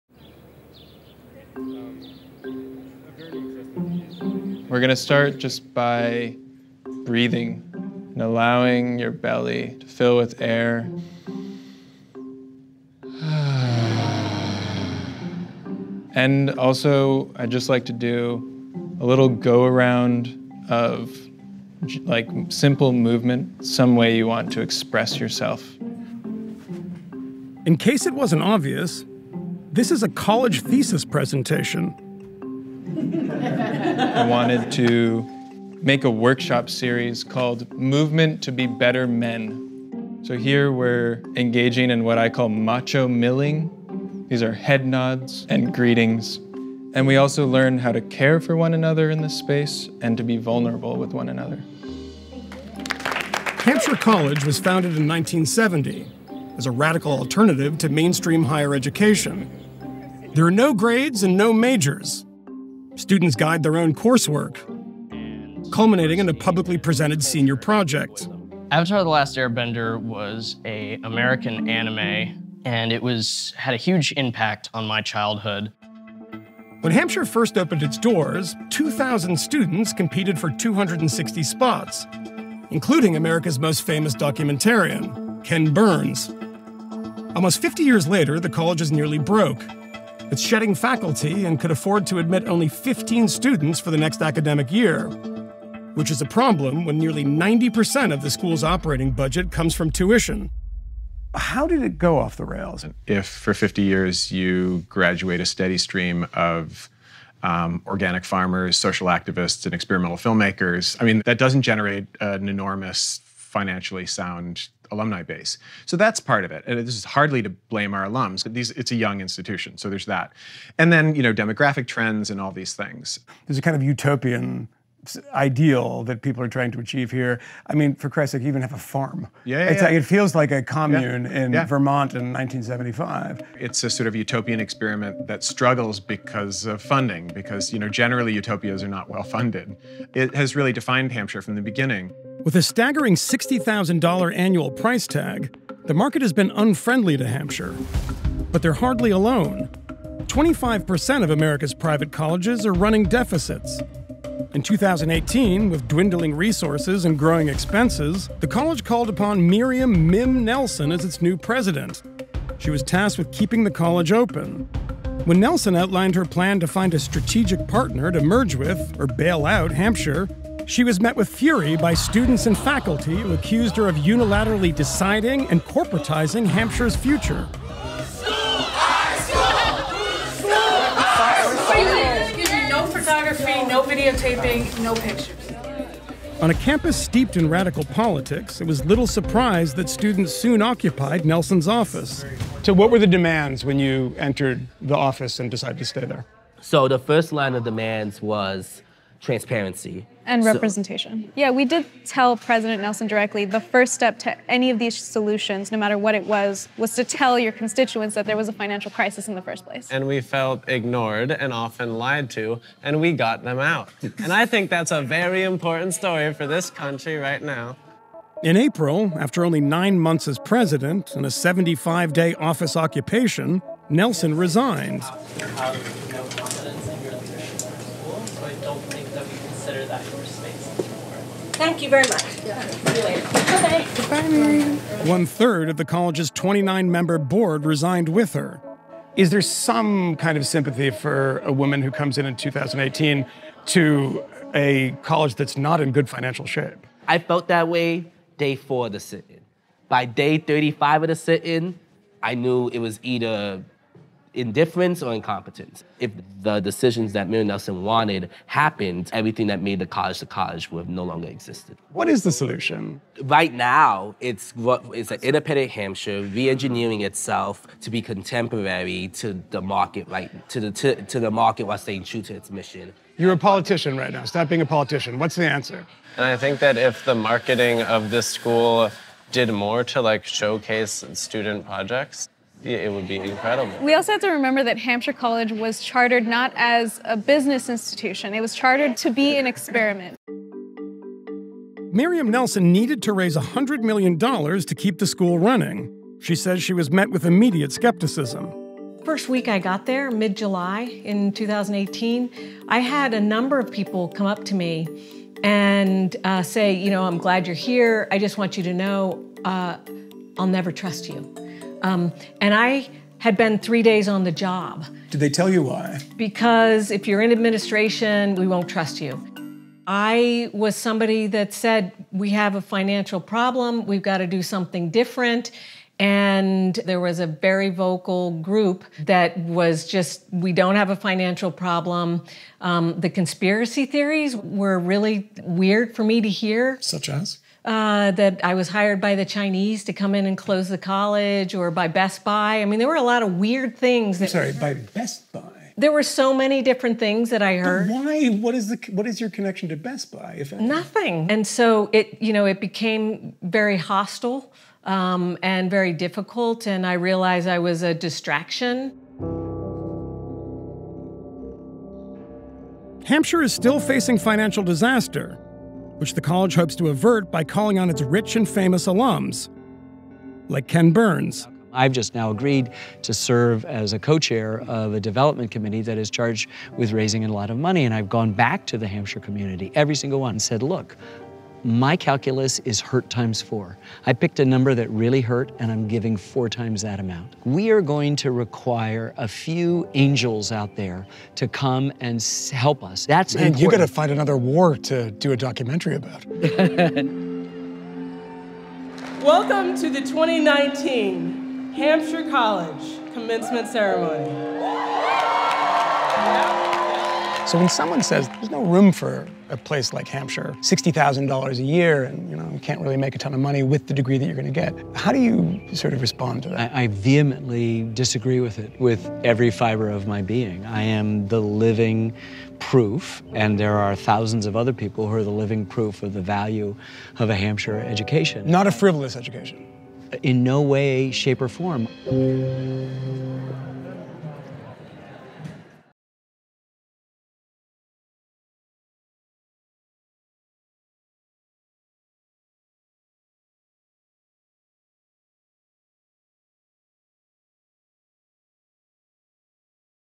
Ken Burns Wants To Save This Liberal Arts College — Before It Dies (HBO)
Hampshire College is in trouble. The liberal arts college in Western Massachusetts was founded in 1970 as a radical experiment in education: there are no grades, and students chart their own coursework. But now the school is finding itself on the verge of bankruptcy and is in danger of shutting down.

Hampshire College costs about $65,000 a year to attend, but only has an endowment of about $48.5 million — which pales in comparison to other, older colleges (neighboring Amherst College has a $2.2 billion dollar endowment, for example). Student enrollment steadily declining over the last few years could mean the death knell for the school, as tuition and fees make up about 90% of their operating budget.